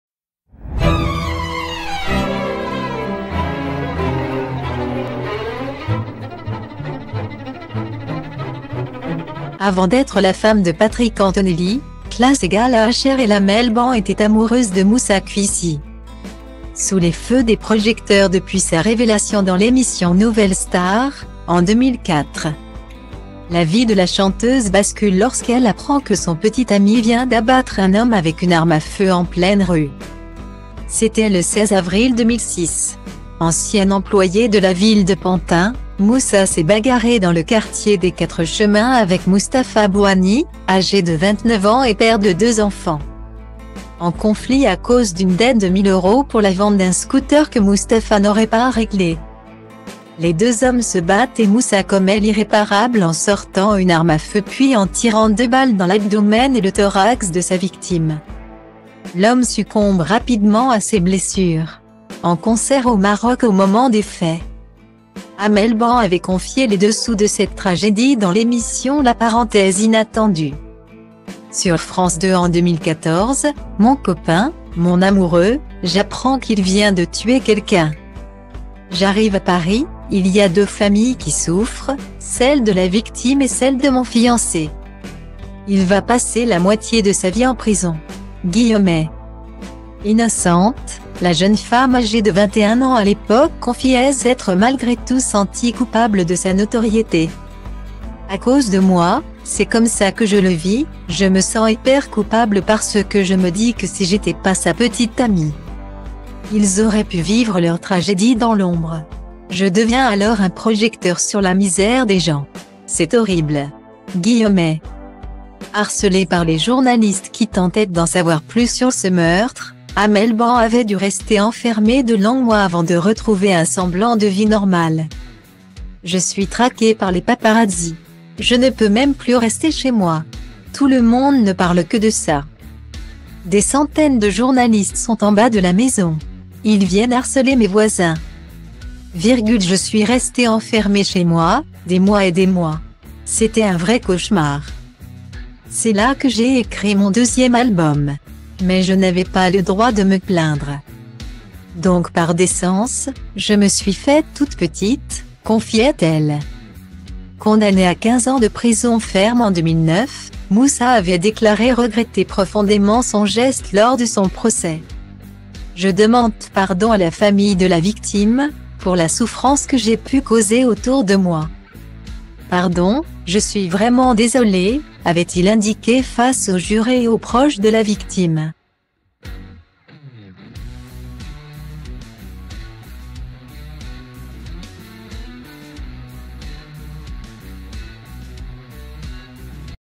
Amel Bent : Son ex-compagnon condamné meurtre, une épreuve dans sa vie
Avant d'être la femme de Patrick Antonelli, class='hrl'Amel Bent était amoureuse de Moussa Kouissi.Sous les feux des projecteurs depuis sa révélation dans l'émission Nouvelle Star (en 2004),la vie de la chanteuse bascule lorsqu'elle apprend que son petit ami vient d'abattre un homme avec une arme à feu en pleine rue.